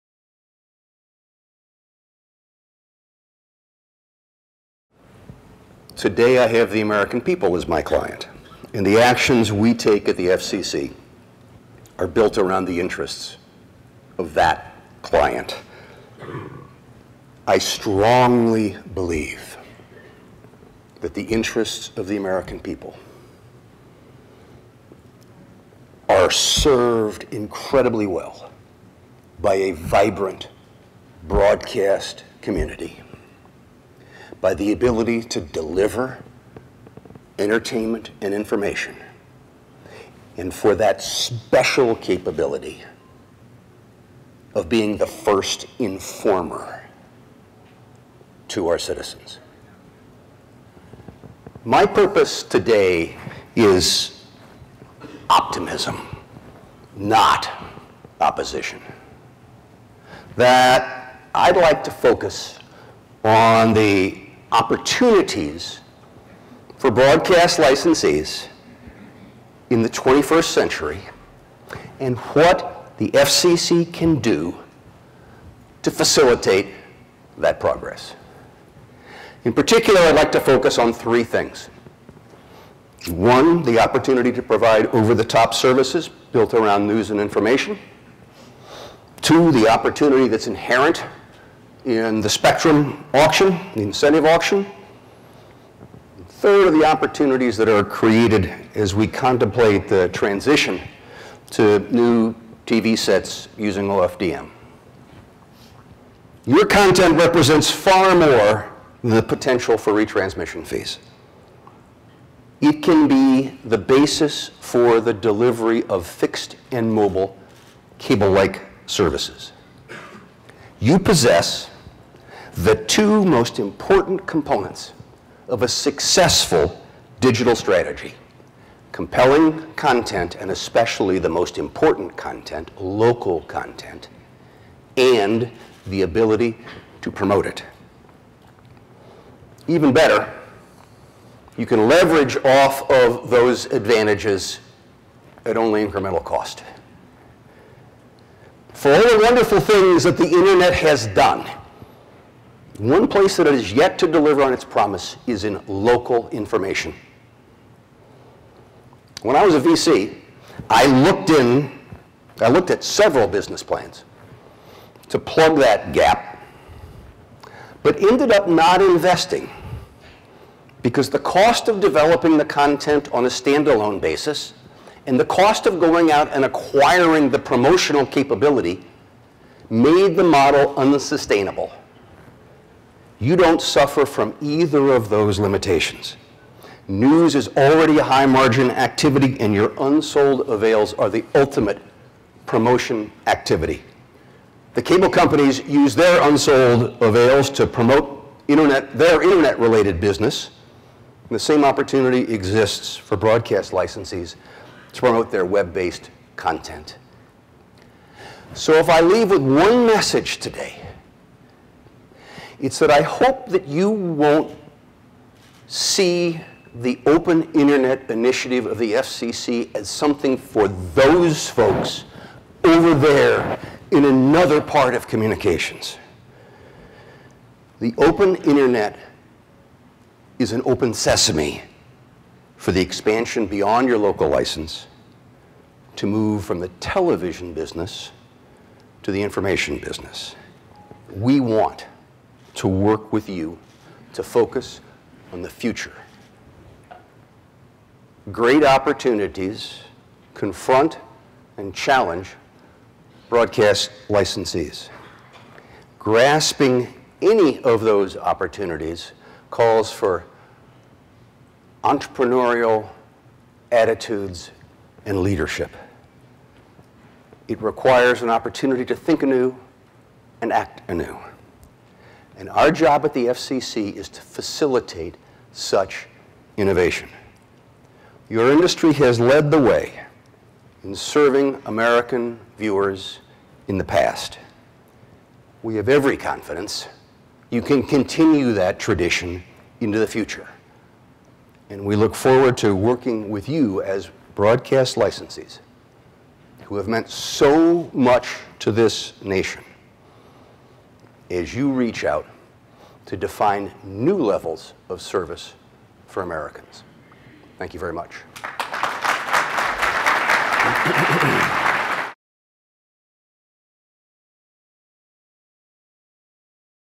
FCC Keynote: Tom Wheeler
FCC Keynote
Date: Tuesday, April 8
9:00 am - 10:00 am

Part of: FCC Keynote

Location: LVH Ballroom A

Marquee Events:
General Sessions

Open To All:
General Sessions

Description:
This is your chance at the NAB Show to get words of wisdom directly from the chairman of the Federal Communications Commission (FCC). Get insights into how FCC Chairman Tom Wheeler views the world of broadcasting and what his expectations are for the coming years.